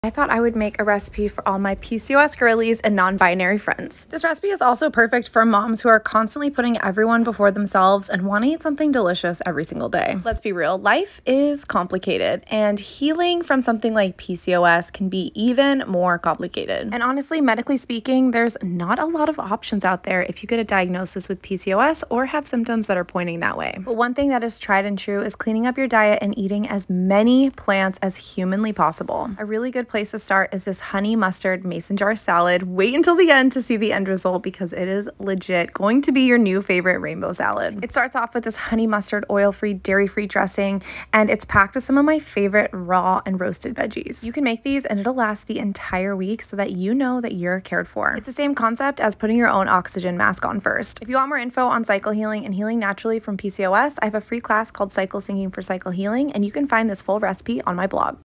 Roasted Veggie Honey Mustard Mason Jar Salads | Oil & Dairy Free | High Protein Meal Prep
This honey mustard mason jar salad is packed with roasted veggies, arugula, cabbage, and the most insane vegan honey mustard dressing. This dressing is also oil-free so it’s perfect for my oil free friends out there or if you’re really looking to clean up your diet.

When making your mason jar salad, be sure to start with dressing on the bottom and greens on top. When you layer it this way, it will stay fresh and delicious for over a week!

This recipe is colorful, flavorful, high in nutrients, and a great meal prep quick meal on the go.  It is the perfect example of eating the rainbow.

This recipe is:

Plant-based
Oil free optional
Dairy free
Great meal prep
A budget friendly recipe
Healthy & nutrient dense